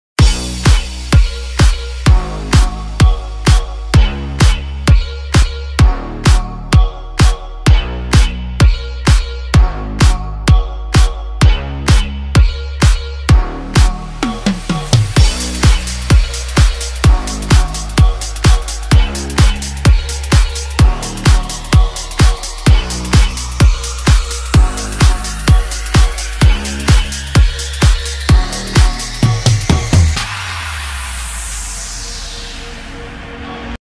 Estrobo de 10 leds de 20w RGB alta potencia
El vídeo muestra el funcionamiento del  Estrobo de 10 leds de 20w RGB alta potencia.